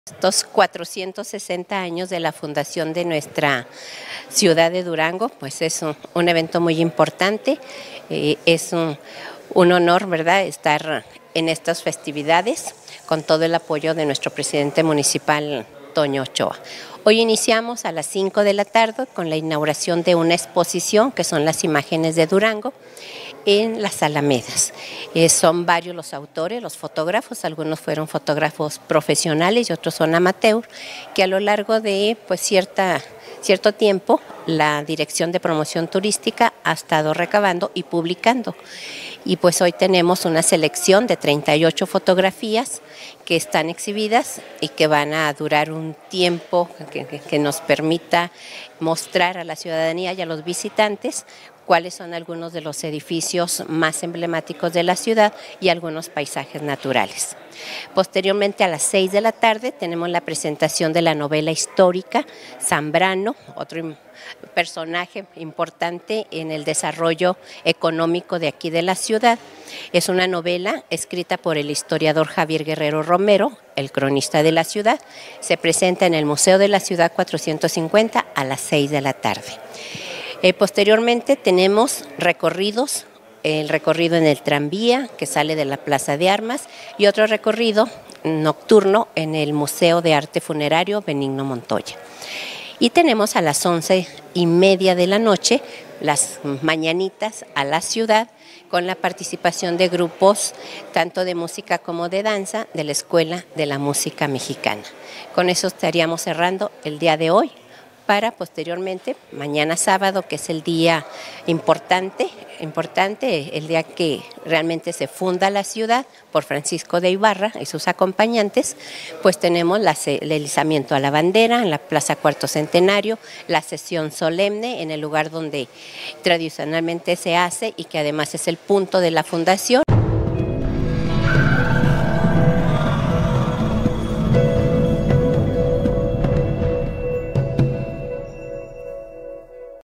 Inician los festejos del 450 aniversario de nuestra ciudad
El día de hoy iniciamos con los festejos del 450 aniversario de nuestra ciudad

Virginia Ruiz Valles
Directora del Instituto Municipal de Arte y Cultura